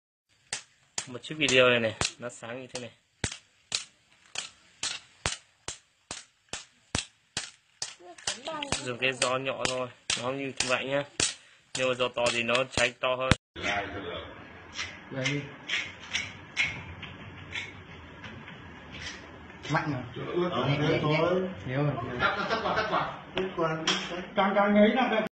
Đá đánh lửa tự nhiên
Đá đánh lửa tự nhiên, Đá lửa có ở đâu? và mua ở đâu? Bạn có thể lên các vùng núi cao ở các bản, vì ở các hang đá hồi xưa hay có loại đá lửa này.
Đá lửa là đá trầm tích giàu silica có cấu trúc tinh thể kín nhỏ và biểu hiện ngoài giống thủy tinh và là một dạng của thạch anh. Đá lửa thông thường có màu xám sẫm, xanh lam, đen hay nâu sẫm. Nó được tìm thấy chủ yếu dưới dạng các cục hay khối trong đá phấn hay đá vôi.